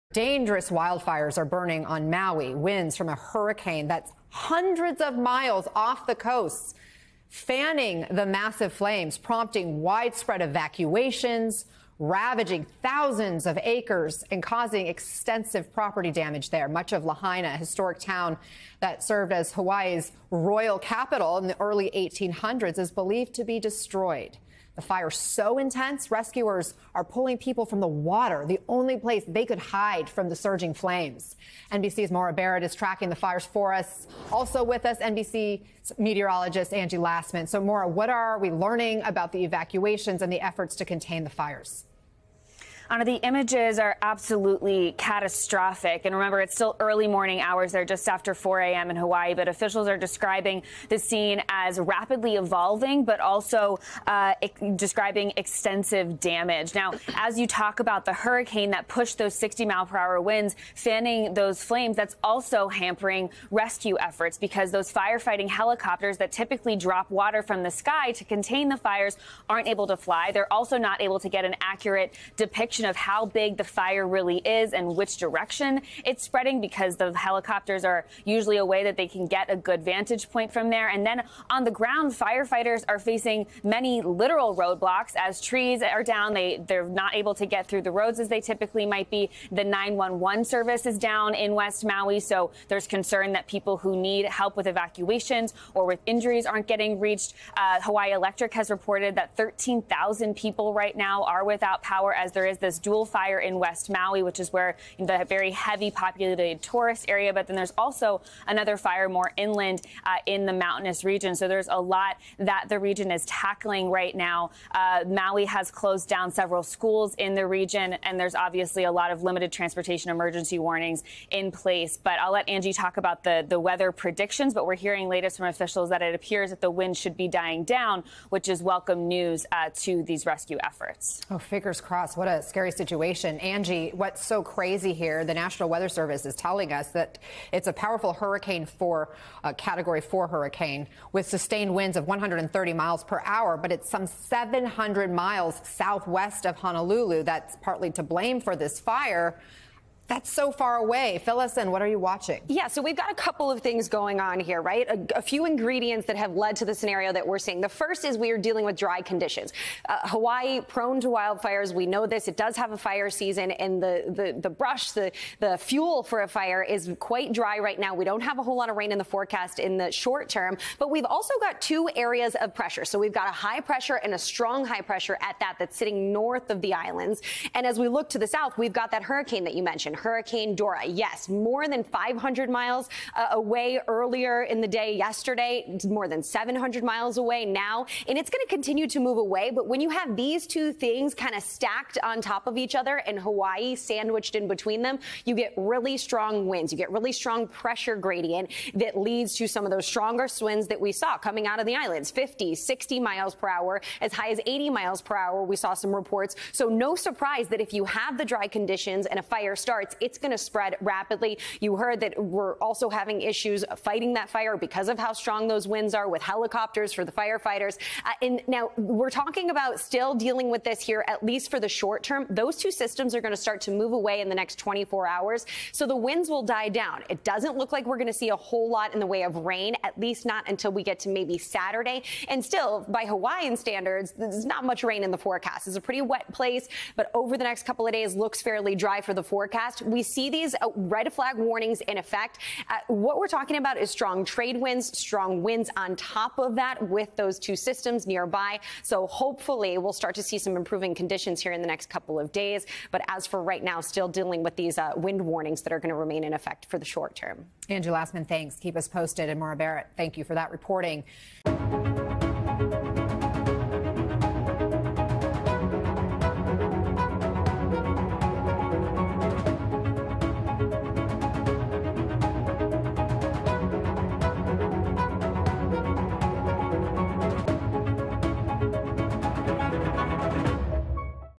Maui wildfires cause extensive damage to island as residents evacuate
Residents on Maui are being evacuated as massive wildfires burn through areas of Hawaii's second largest island. NBC News' Maura Barrett and Angie Lassman report on how the wildfires are being fanned by Hurricane Dora and how firefighters are working to contain the blaze.